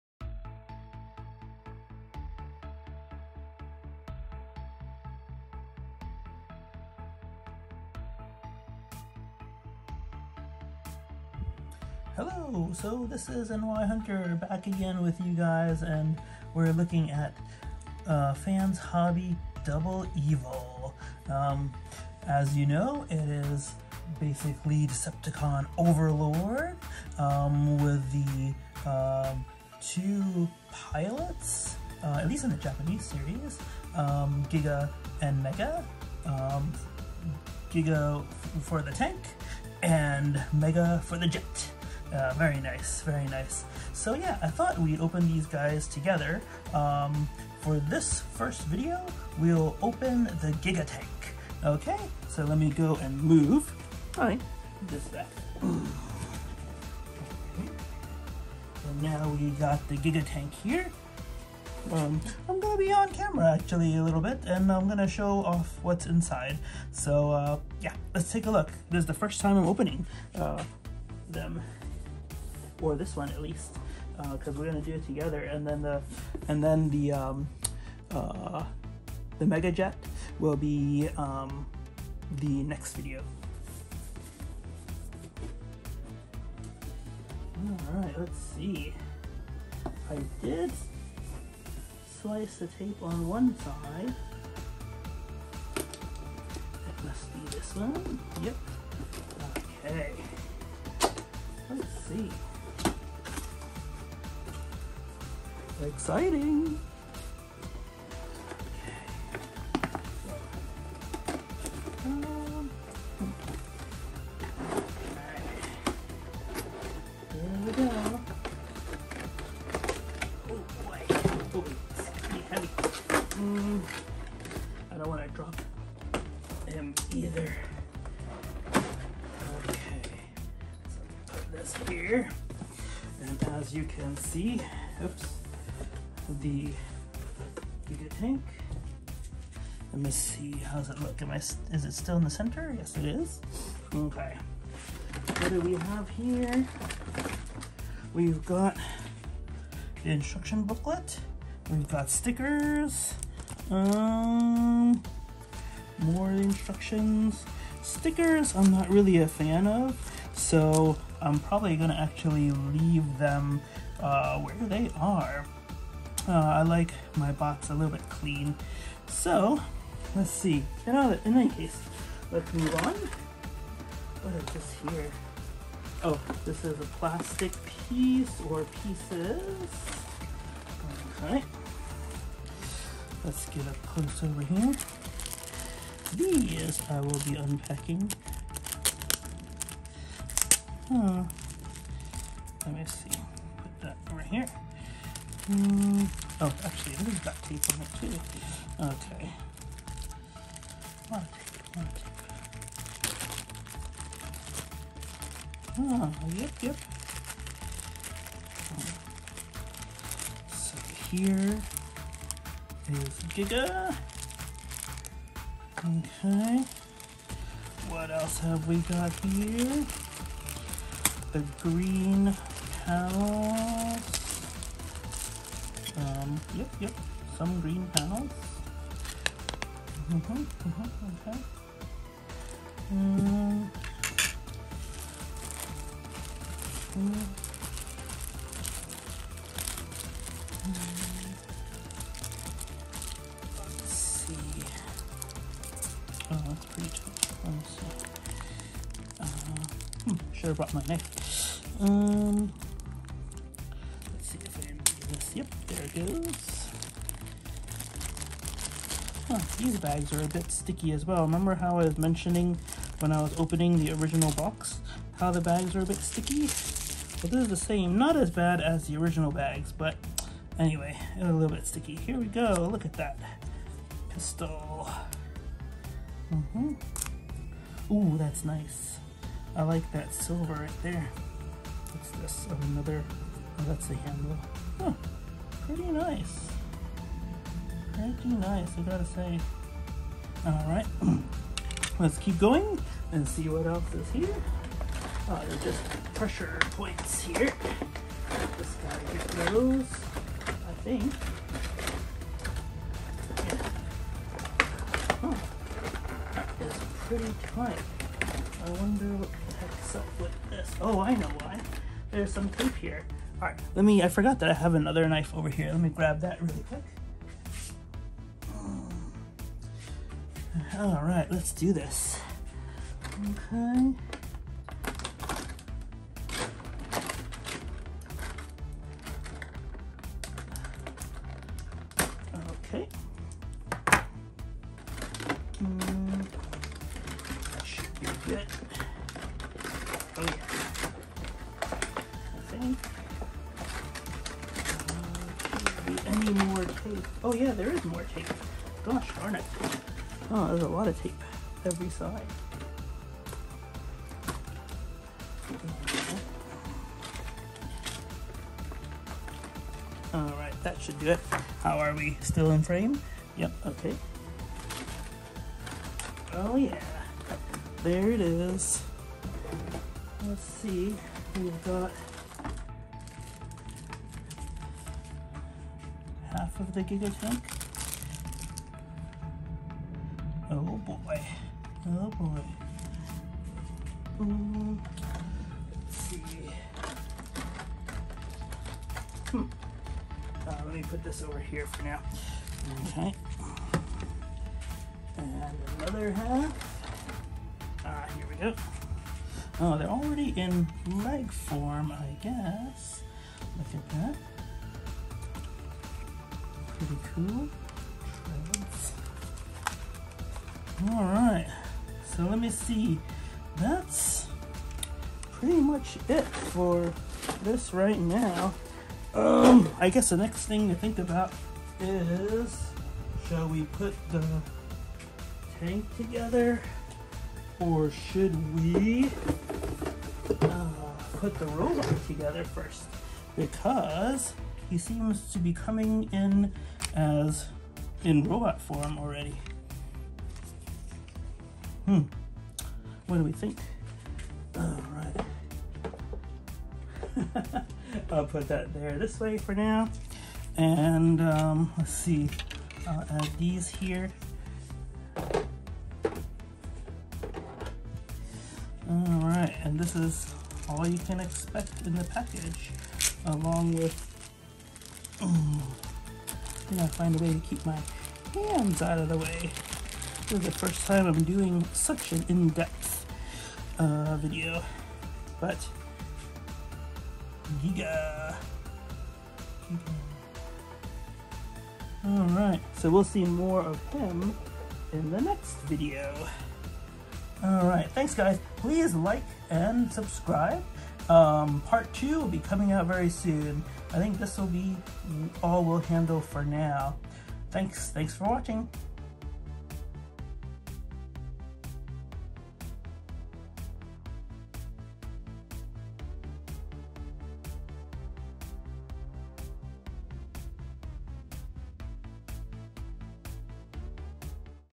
Fans Hobby Double Evil (Overlord) GigaTank Unboxing
Fans Hobby Double Evil GigaTank Opening
So excited to open this up and see what's inside :D

Video 1